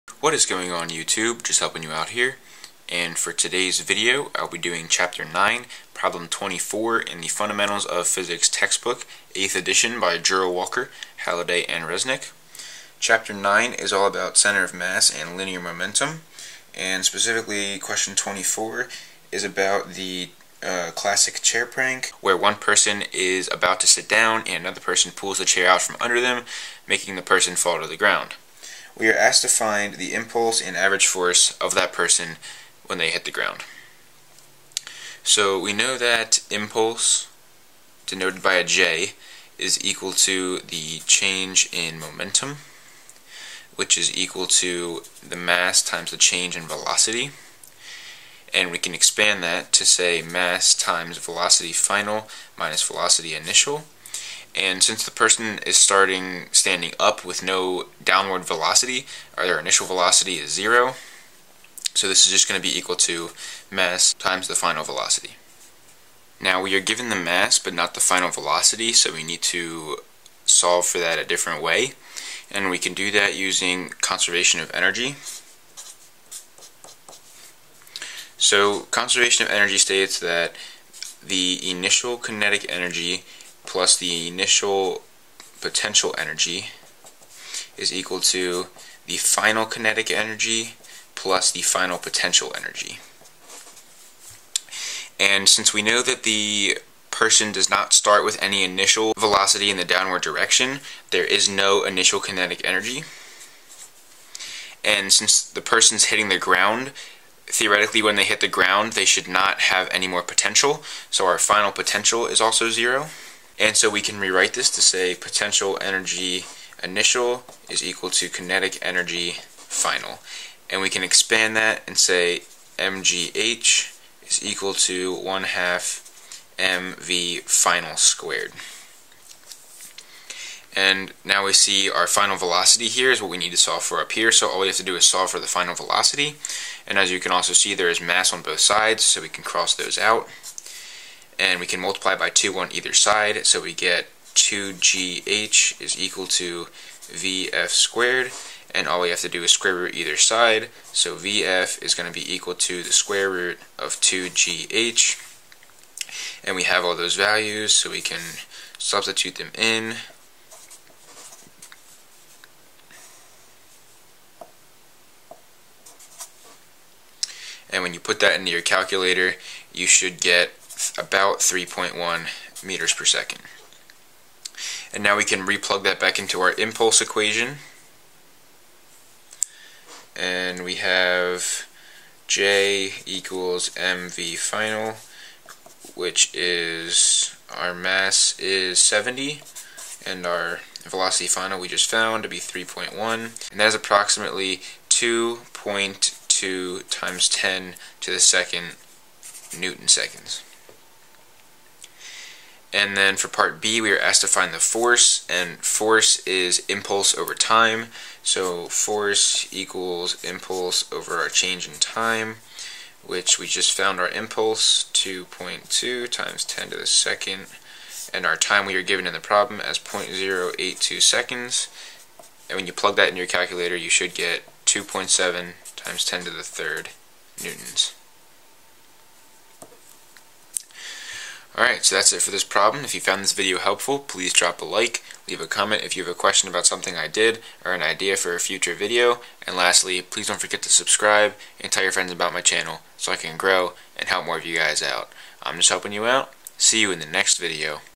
Fundamentals of Physics 8th Edition (Walker/Halliday/Resnick), Chapter 9, Problem 24 Solution
This is my solution to problem 24 in chapter 9 of Fundamentals of Physics 8th Edition textbook by Walker, Halliday, and Resnick.